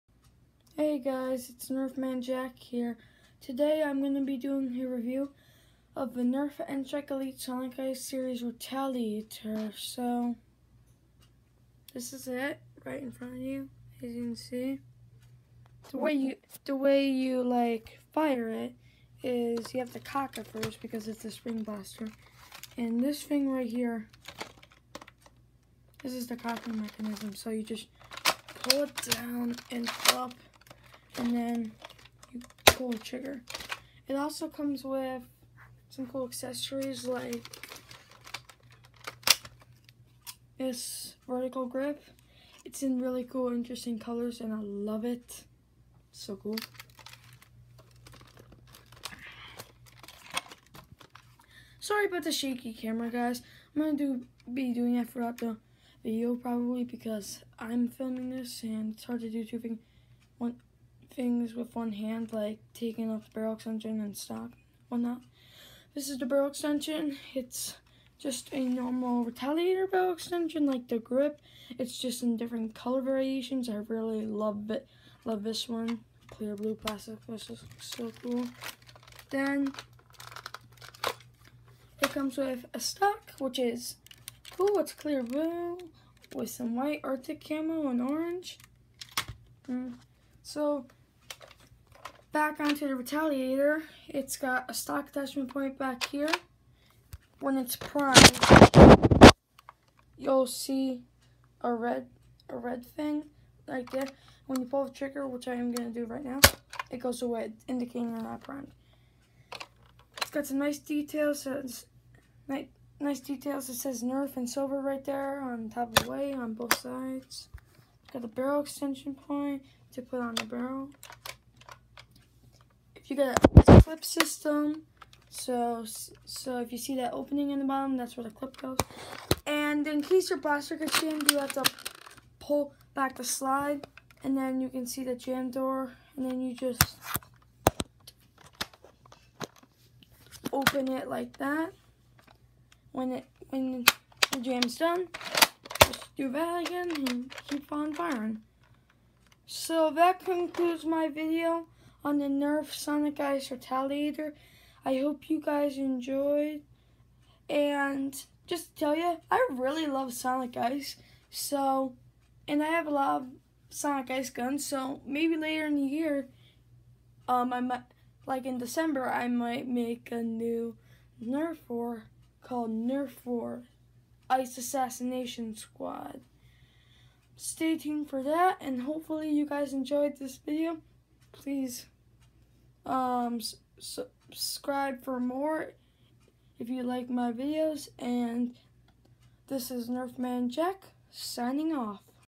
Nerf sonic ice retaliator review
Hey guys today I'm doing a review of the sonic ice retaliator and I will probably be posting a trailer for nerf war Ice assassination squad around when it gets cold probably in like late November hope you enjoyed the video more vids are coming soon so subscribe to see them anyways see ya then